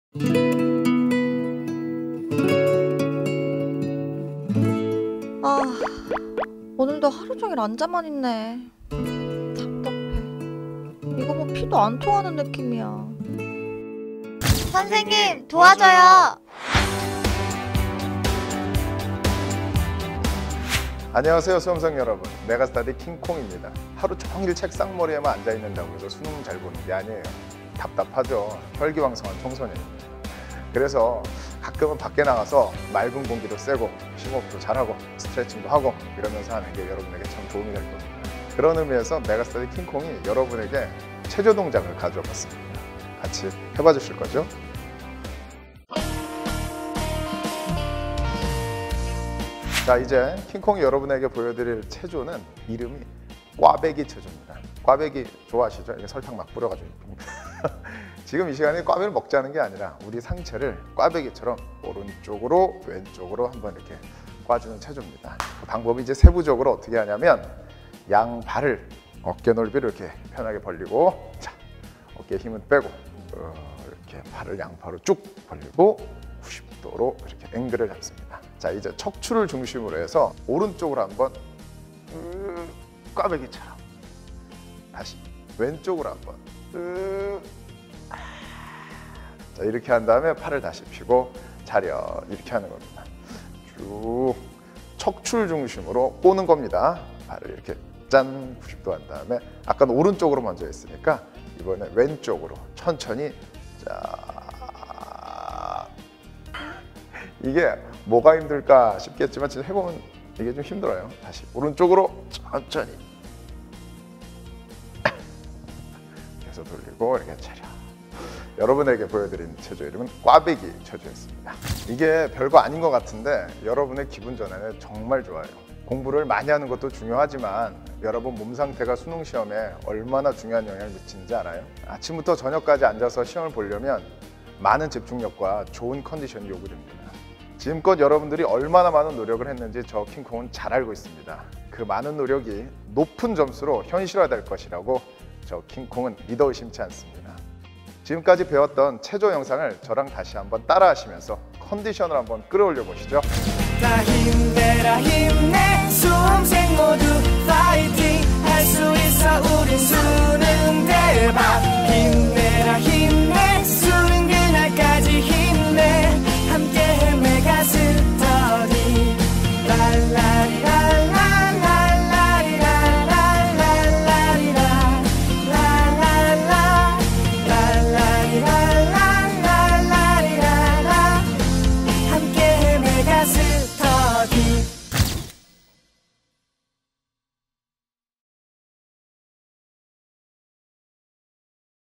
[메가스터디] 영어 킹콩 "수능 D-15 컨디션을 끌어올릴 시간 _ 꽈배기 체조"
공부는 앉아서만 하는게 아닙니다! 가끔은 일어나서 몸 풀면서 하세요! - 합격불변의 법칙 메가스터디